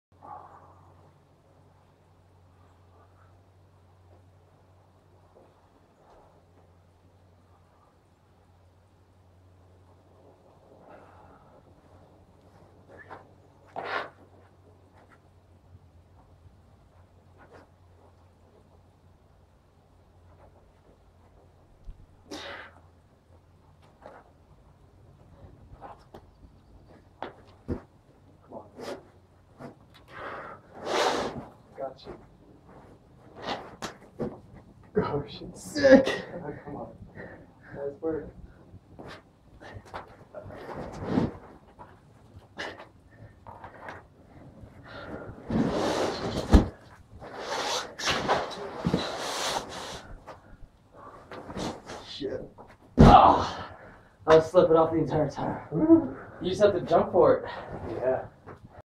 Stone On Bone
Aquarian Valley, Ca. Stoner Cave(FA: Me) V4-6? Hang and drop problem, future topouts go (high) left and (short) right.